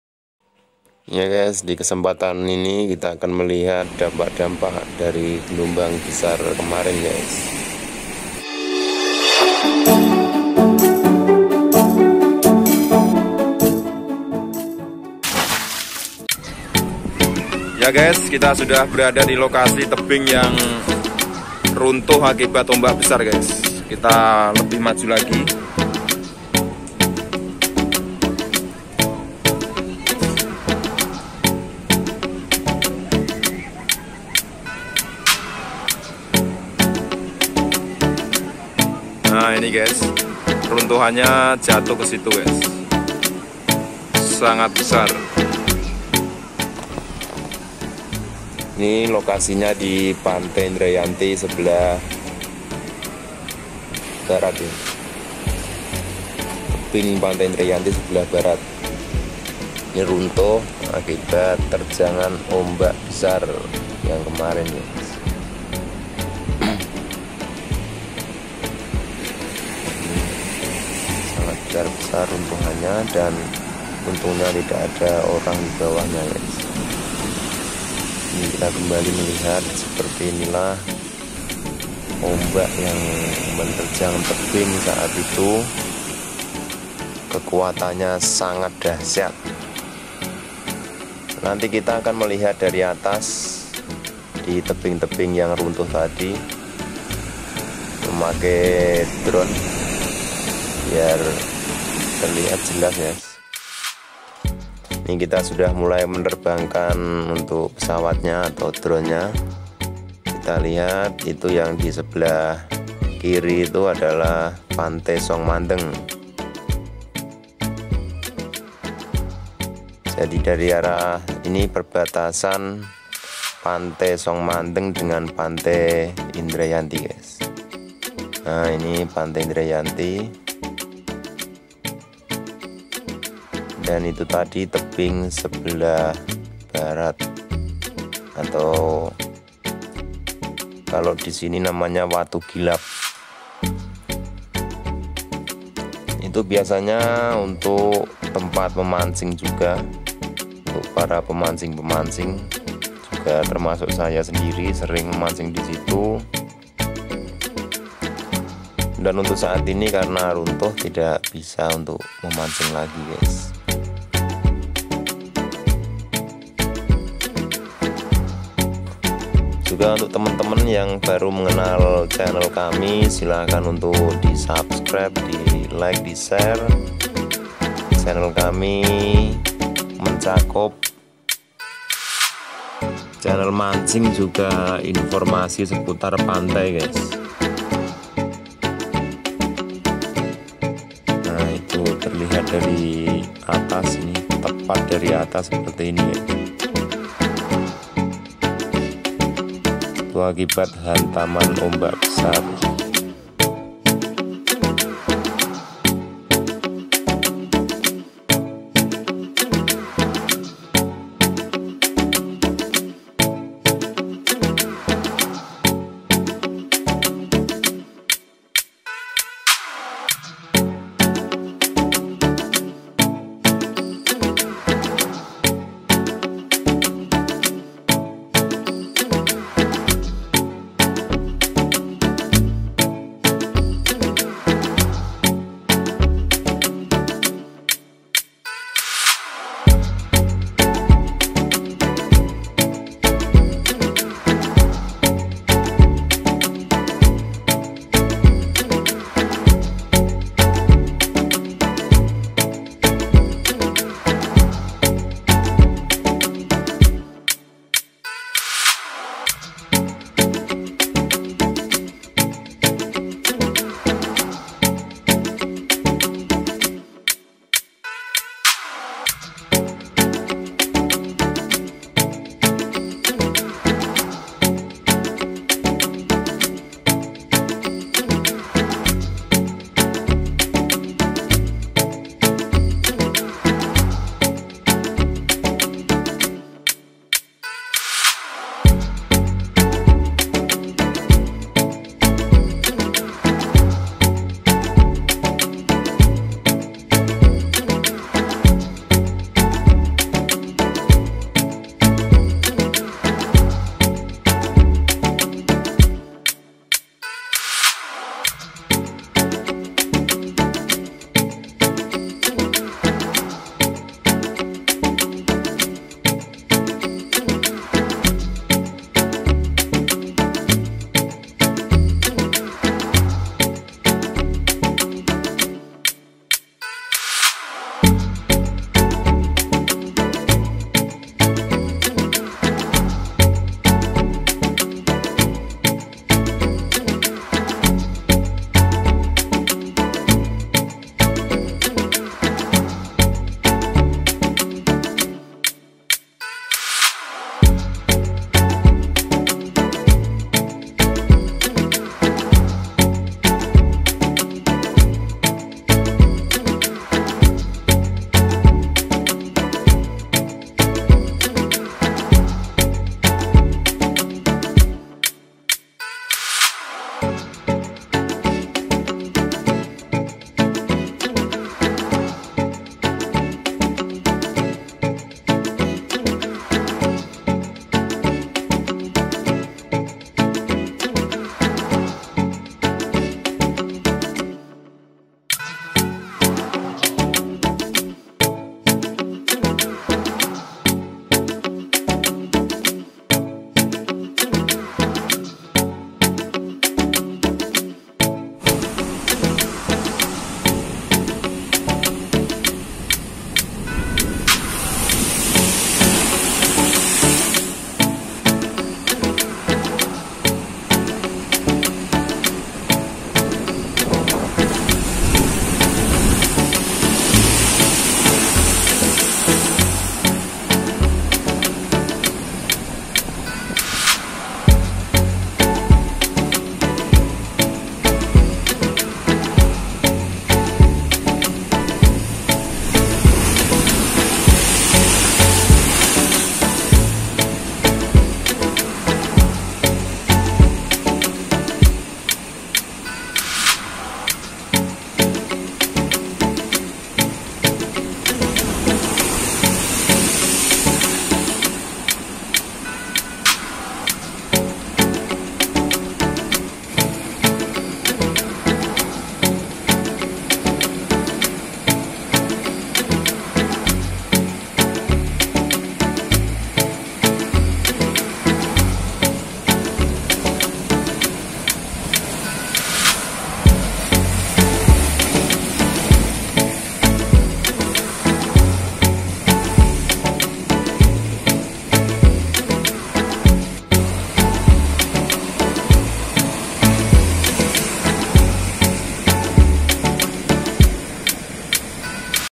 AKIBAT GELOMBANG BESAR TEBING SAMPAI RUNTUH
tebing di pantai indrayanti runtuh akibat ombak besar kemarin